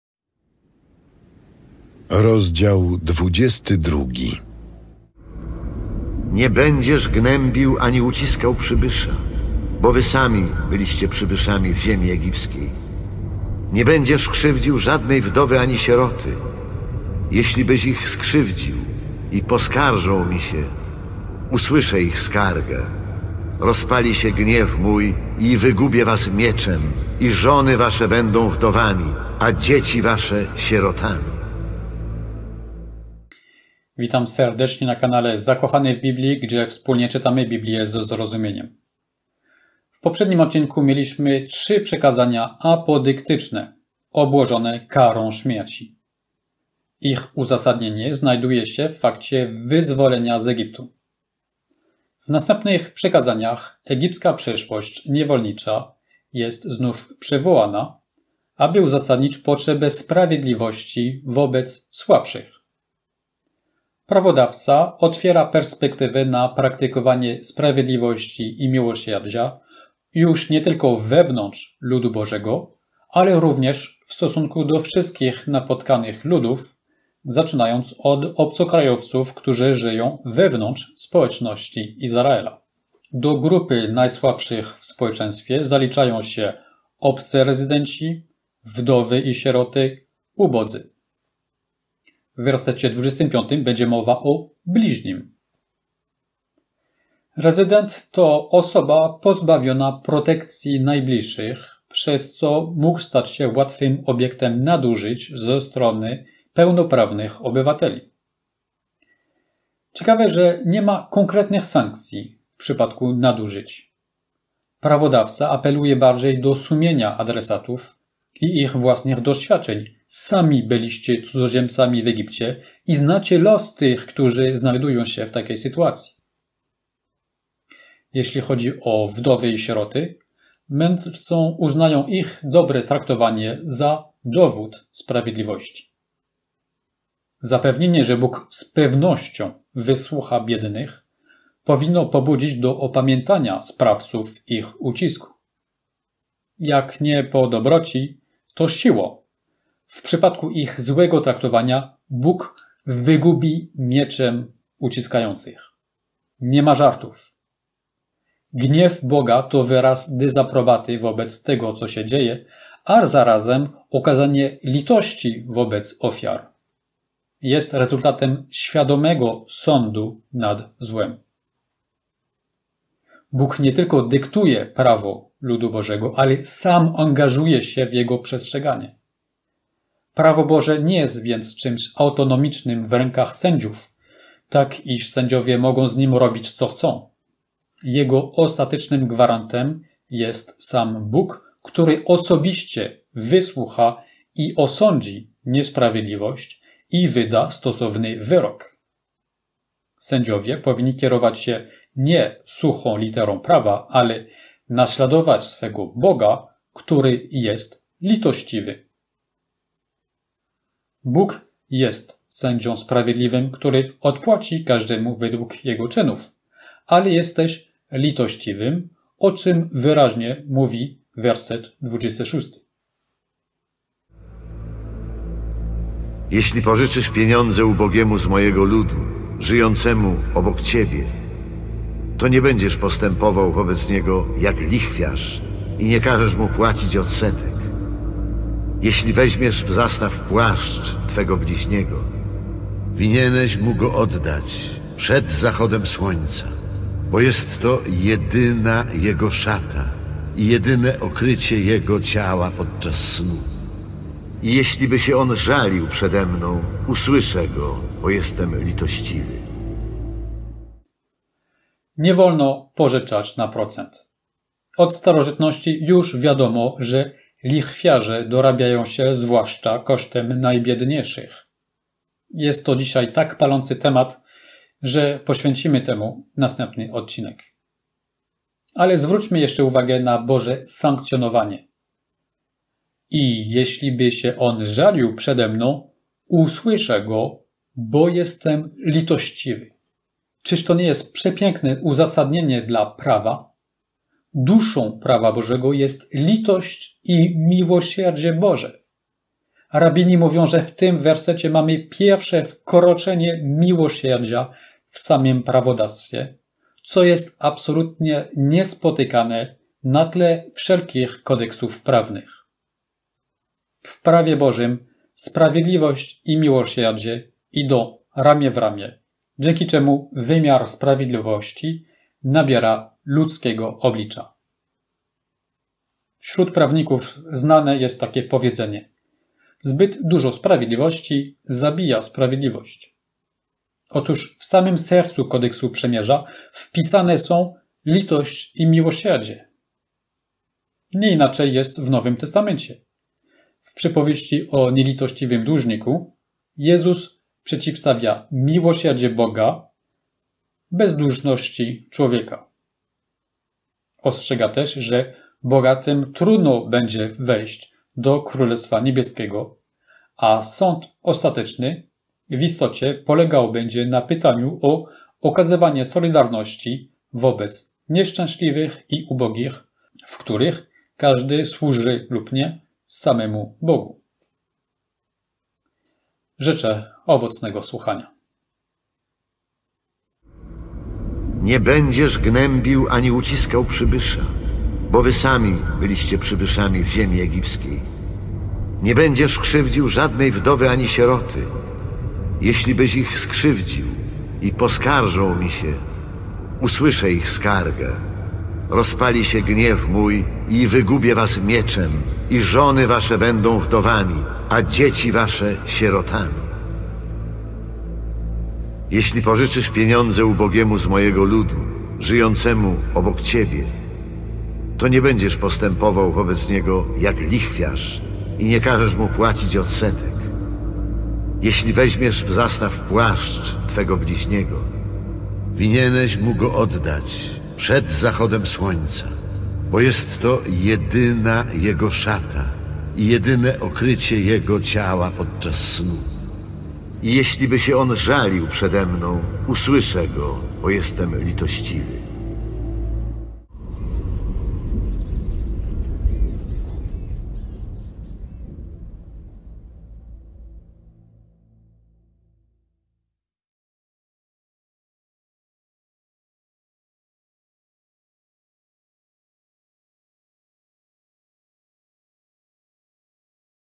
Troska o biednych (Wj 22, 20-26)
Nie będziesz gnębił ani uciskał przybysza, bo wy sami byliście przybyszami w ziemi egipskiej. Nie będziesz krzywdził żadnej wdowy ani sieroty. Jeślibyś ich skrzywdził i poskarżą Mi się, usłyszę ich skargę, rozpali się gniew mój, i wygubię was mieczem; i żony wasze będą wdowami, a dzieci wasze sierotami. (Wj 22, 20-23)
W poprzednim odcinku mieliśmy trzy przykazania apodyktyczne obłożone karą śmierci. Ich uzasadnienie znajduje się w fakcie wyzwolenia z Egiptu. W następnych przykazaniach egipska przeszłość niewolnicza jest znów przywołana, aby uzasadnić potrzebę sprawiedliwości wobec słabszych. Prawodawca otwiera perspektywę na praktykowanie sprawiedliwości i miłosierdzia już nie tylko wewnątrz Ludu Bożego, ale również w stosunku do wszystkich napotkanych ludów, zaczynając od obcokrajowców, którzy żyją wewnątrz społeczności Izraela.
Do grupy najsłabszych w społeczeństwie zaliczają się obcy rezydenci, wdowy i sieroty, ubodzy. W wersecie 25 będzie mowa o „bliźnim”. Rezydent to osoba pozbawiona protekcji najbliższych, przez co mógł stać się łatwym obiektem nadużyć ze strony pełnoprawnych obywateli. Ciekawe, że nie ma konkretnych sankcji w przypadku nadużyć. Prawodawca apeluje bardziej do sumienia adresatów i ich własnych doświadczeń: “sami byliście cudzoziemcami w Egipcie i znacie los tych, którzy znajdują się w takiej sytuacji”. Jeśli chodzi o wdowy i sieroty, mędrcy uznają ich dobre traktowanie za dowód sprawiedliwości.
Zapewnienie, że Bóg „z pewnością” wysłucha biednych powinno pobudzić do opamiętania sprawców ich ucisku. Jak nie po dobroci, to siłą: w przypadku ich złego traktowania, Bóg „wygubi mieczem” uciskających – nie ma żartów! Gniew Boga to wyraz dezaprobaty wobec tego co się dzieje, a zarazem okazanie litości wobec ofiar. Jest rezultatem świadomego sądu nad złem.
Bóg nie tylko dyktuje prawo Ludu Bożego, ale i sam angażuje się w jego przestrzeganie. Prawo Boże nie jest więc czymś autonomicznym w rękach sędziów, tak iż sędziowie mogą z nim robić, co chcą. Jego ostatecznym gwarantem jest sam Bóg, który osobiście wysłucha i osądzi niesprawiedliwość i wyda stosowny wyrok. Sędziowie powinni kierować się nie suchą literą prawa, ale naśladować swego Boga, który jest litościwy.
Bóg jest sędzią sprawiedliwym, który odpłaci każdemu według jego czynów, ale jest też litościwym, o czym wyraźnie mówi werset 26:
Jeśli pożyczysz pieniądze ubogiemu z mojego ludu, żyjącemu obok ciebie, to nie będziesz postępował wobec niego jak lichwiarz i nie każesz mu płacić odsetek.
Jeśli weźmiesz w zastaw płaszcz twego bliźniego, winieneś mu go oddać przed zachodem słońca, bo jest to jedyna jego szata i jedyne okrycie jego ciała podczas snu. I jeśliby się on żalił przede Mną, usłyszę go, bo jestem litościwy. (Wj 22, 24-26)
Nie wolno pożyczać na procent. Od starożytności już wiadomo, że lichwiarze dorabiają się zwłaszcza kosztem najbiedniejszych! Jest to dzisiaj tak palący temat, że poświęcimy temu następny odcinek.
Ale zwróćmy jeszcze uwagę na Boże sankcjonowanie: „I jeśliby się on żalił przede Mną, usłyszę go, bo jestem litościwy.” - czyż to nie jest przepiękne uzasadnienie dla Prawa? Duszą Prawa Bożego jest litość i miłosierdzie Boże! Rabini mówią, że w tym wersecie mamy pierwsze wkroczenie miłosierdzia w samym prawodawstwie, co jest absolutnie niespotykane na tle wszelkich kodeksów prawnych! W Prawie Bożym sprawiedliwość i miłosierdzie idą ramię w ramię, dzięki czemu wymiar sprawiedliwości nabiera ludzkiego oblicza. Wśród prawników znane jest takie powiedzenie: „Zbyt dużo sprawiedliwości zabija sprawiedliwość”. Otóż w samym sercu Kodeksu Przymierza wpisane są litość i miłosierdzie!
Nie inaczej jest w Nowym Testamencie. W przypowieści o nielitościwym dłużniku (Mt 18, 23-35) Jezus przeciwstawia miłosierdzie Boga bezduszności człowieka; ostrzega też, że bogatym trudno będzie wejść do królestwa niebieskiego (Mt 19, 23-26), a sąd ostateczny w istocie polegał będzie na pytaniu o okazywanie solidarności wobec nieszczęśliwych i ubogich, w których każdy służy (lub nie) samemu Bogu (Mt 25, 31-46).

Biblia Pięcioksiąg Tora
Stary Testament Tanach
Księga Wyjścia Szemot Exodus
Mojżesz Aaron faraon
Judaizm Religia żydowska Żydzi